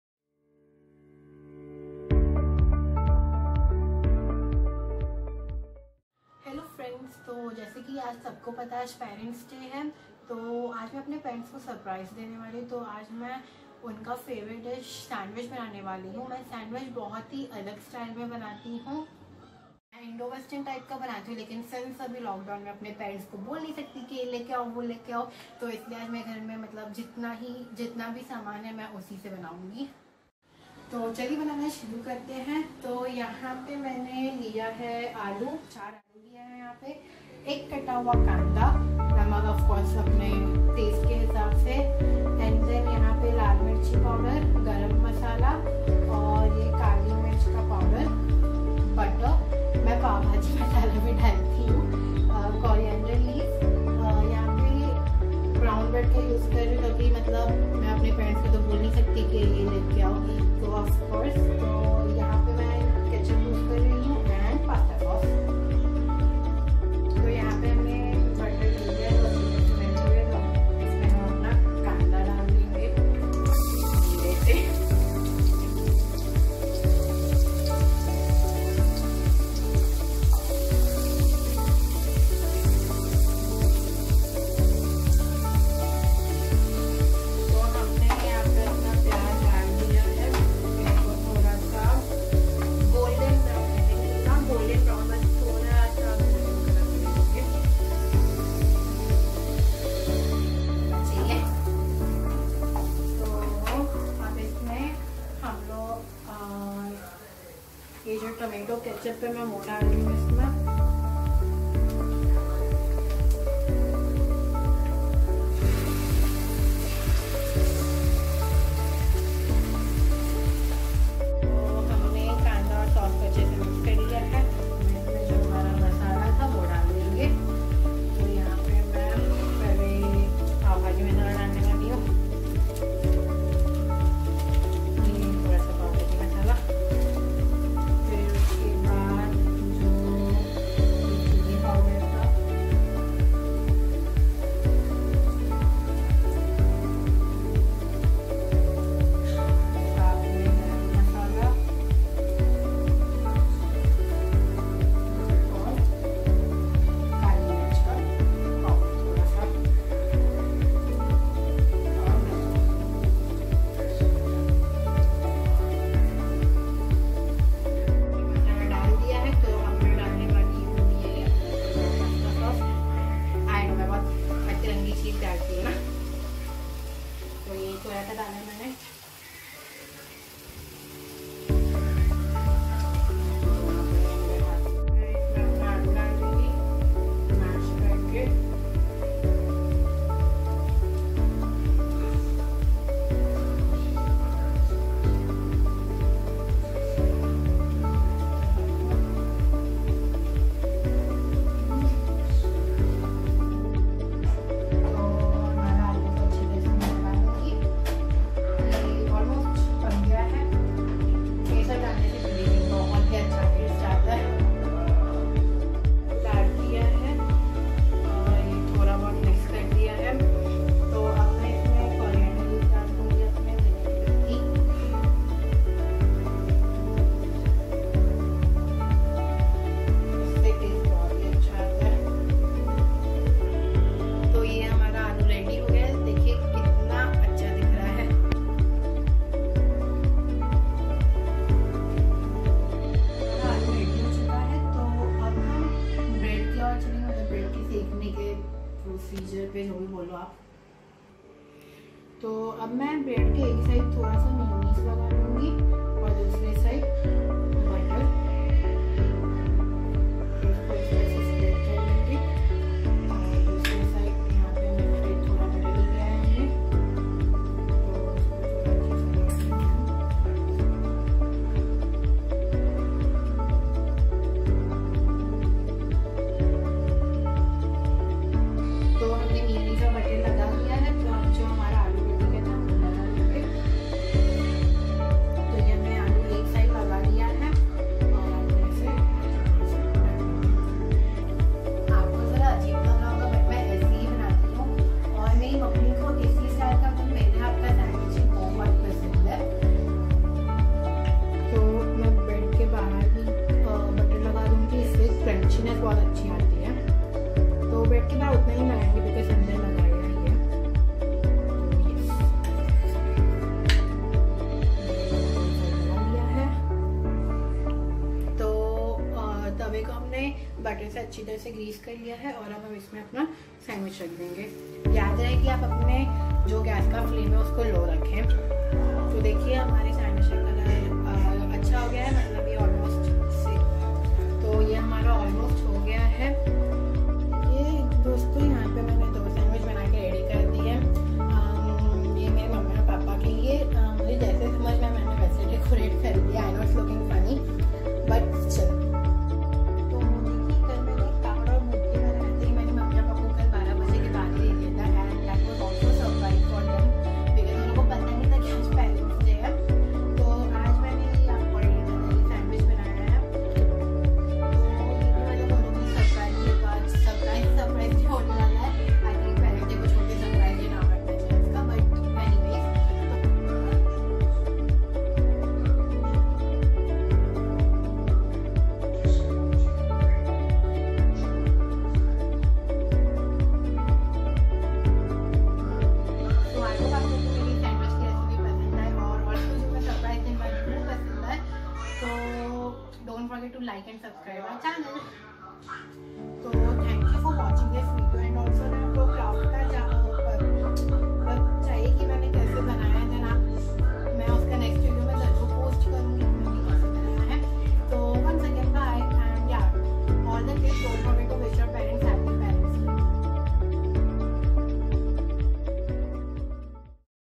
Parents day surprise# My daughter# card# yummy sandwich
She makes amazing sandwich. it was very tasty...u should try it. very beautiful card...made my day